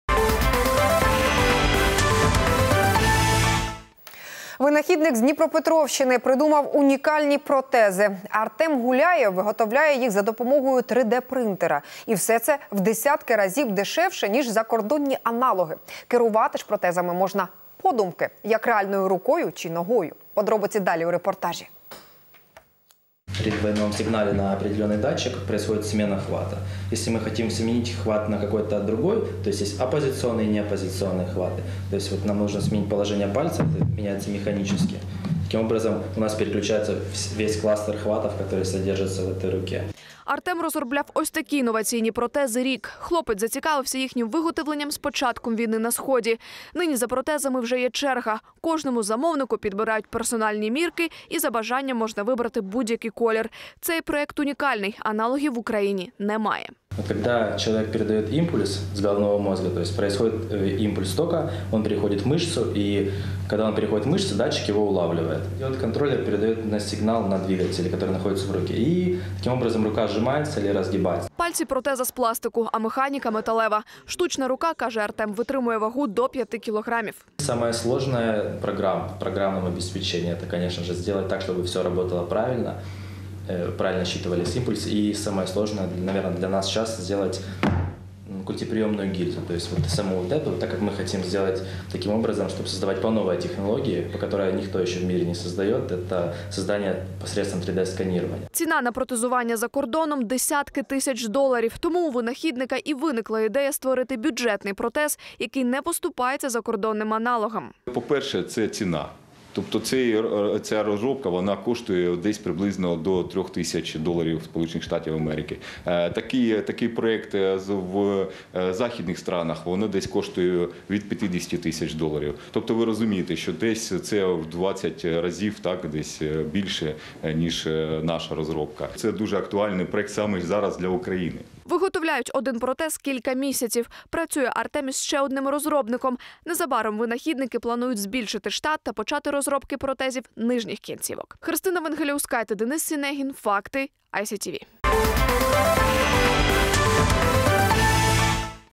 Украинские бионические протезы: управляются силой мысли и в 16 раз дешевле аналогов
Уникальные бионические протезы разрабатывает изобретатель в Днепропетровской области. А делает он это с помощью 3-D принтера. Протезы не уступают в практичности и функциональности зарубежным аналогам. А стоят в разы дешевле. Управлять рукой-протезом можно с помощью силы мысли.